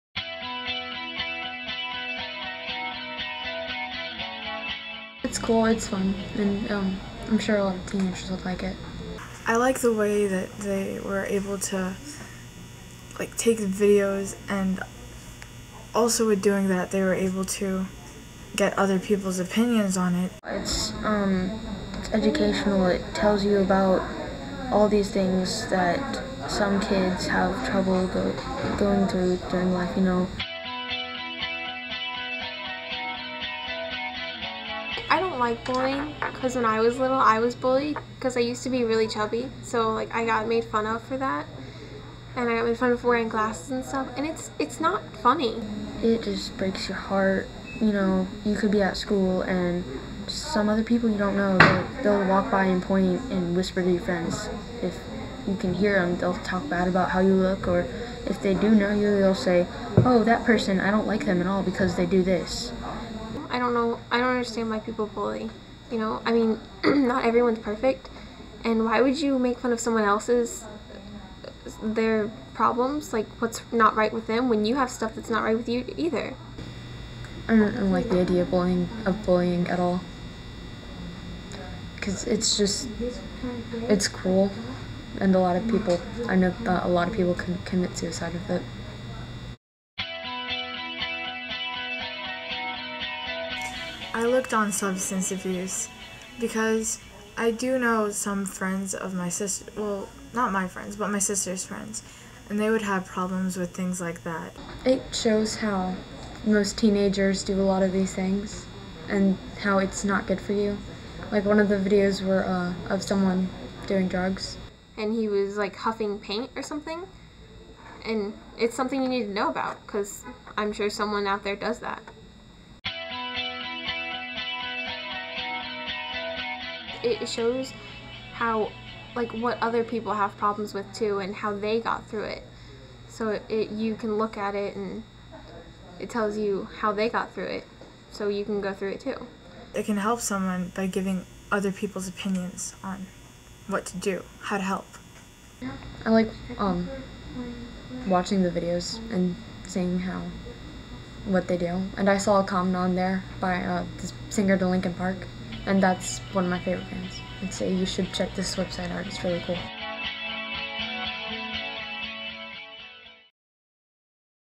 Cafe Nights at the Library
A group of teens gathered at the public library in Fernandina Beach, Florida. After enjoying pizza and soft drinks, they watched In Search of Me Cafe videos on a big screen then explored the site on library PC's. Here are some of their comments about